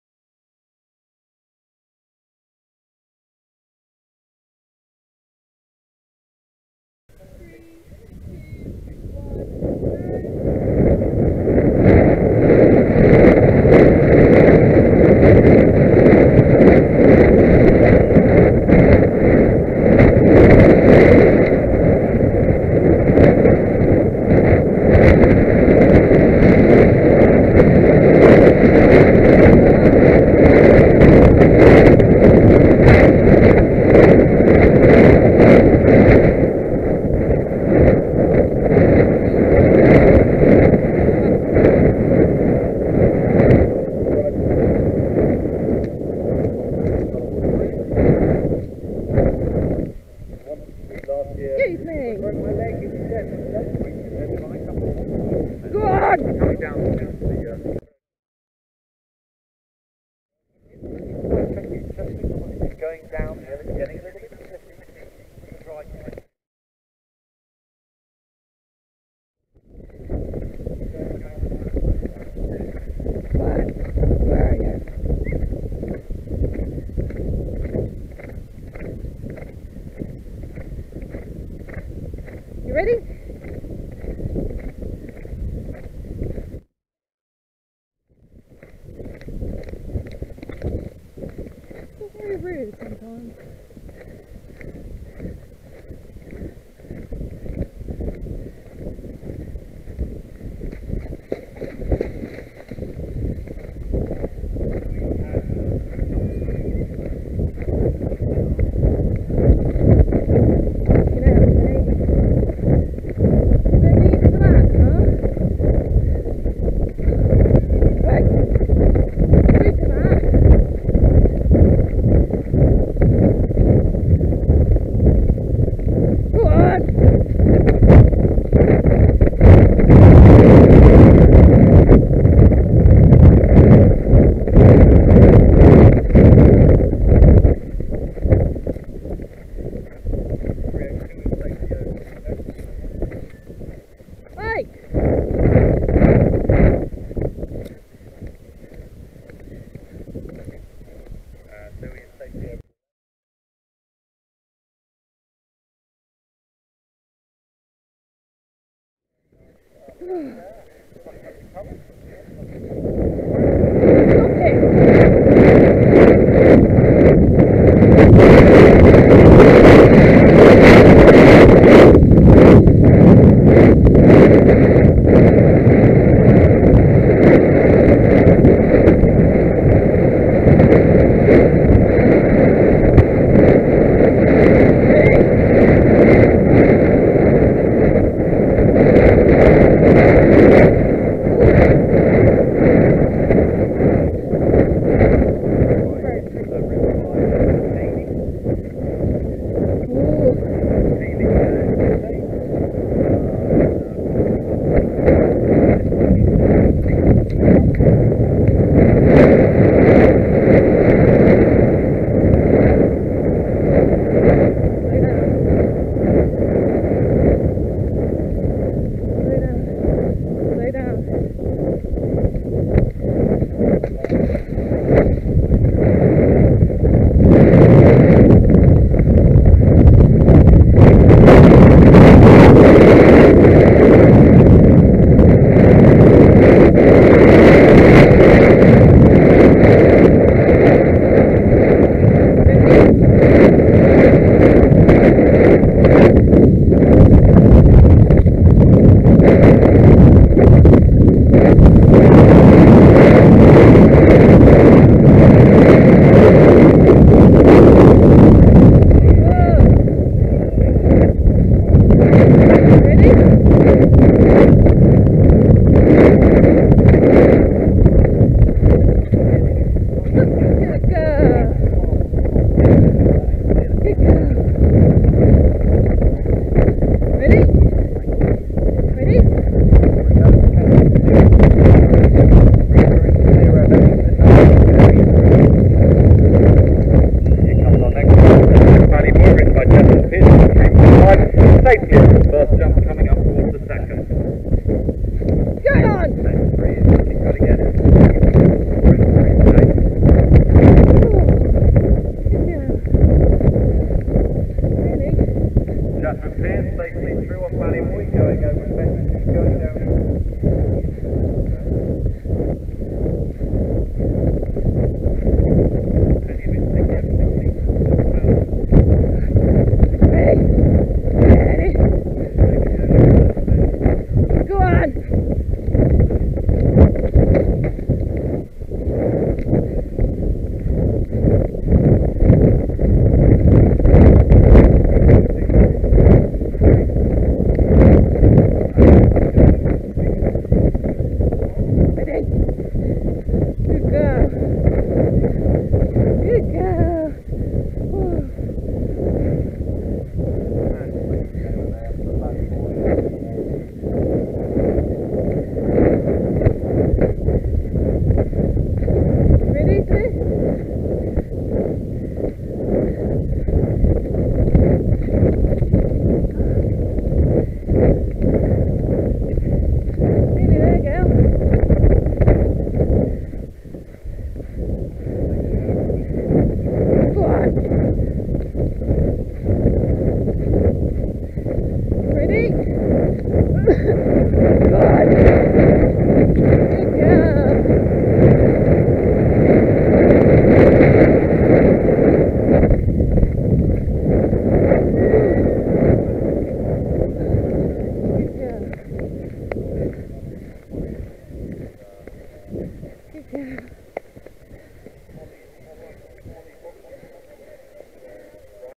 Firle Hunter Trials 2014 (Headcam)
Mine and Pennys' first Firle Hunter Trial, was a brilliant day but with a few iffy moments including backing off most of the jumps, spooking at a pile of branches that we rode past, a bit of running sideways, unexpectedly leaping the water and an extremely near run out! Still managed a clear round though! :) silly old mare x